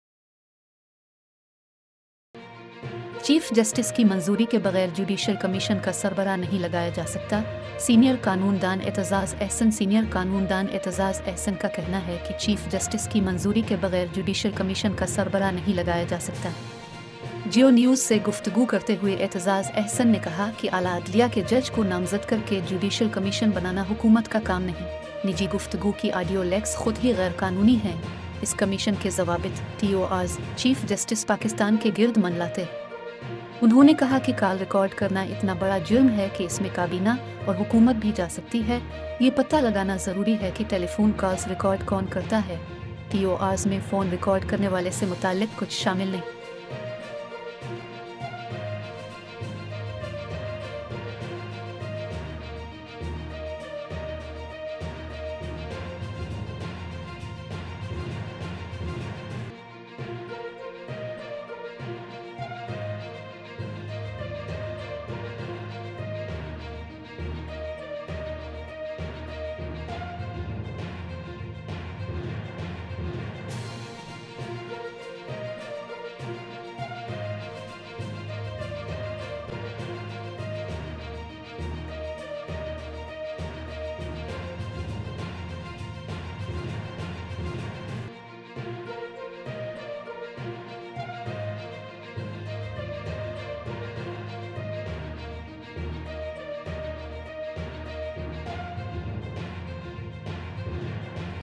The head of the Judicial Commission cannot be appointed without the approval of the Chief Justice,
.Aitzaz Ahsan